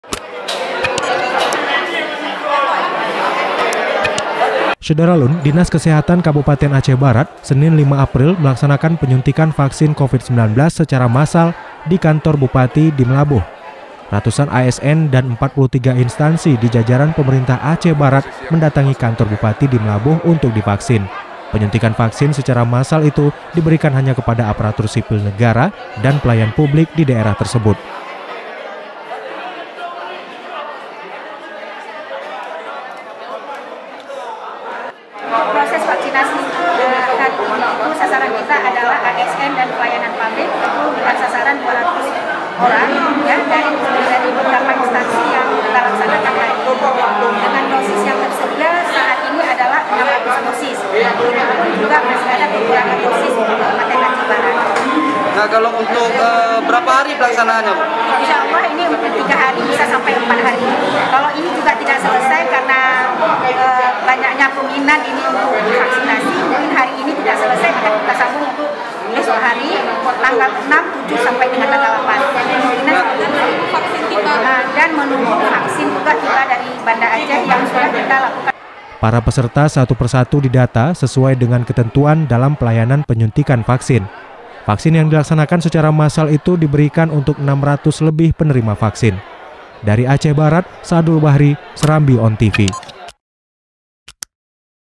Ratusan ASN di Aceh Barat Disuntik Vaksin Covid-19
SERAMBITV.COM, MEULABOH – Dinas Kesehatan (Dinkes), Kabupaten Aceh Barat, Senin (5/4/2021) melaksanakan penyuntikan vaksin Coronavirus Disease (Covid-19), secara massal yang di Kantor Bupati di Meulaboh.

Ratusan ASN dari 43 instansi di jajaran Pemerintah Aceh Barat mendatangi kantor bupati di Meulaboh untuk divaksin.

Penyuntikan vaksin secara masal tersebut diberikan hanya kepada Aparatur Sipil Negara (ASN), dan pelayan publik di daerah tersebut.

Para peserta secara satu persatu di data, sesuai dengan ketentuan dalam pelayanan penyuntikan vaksin tersebut.

Vaksin yang dilaksanakan secara masal itu diberikan untuk 600 lebih penerima vaksin.

Narator: Ilham
Video Editor: M Anshar @Aanpix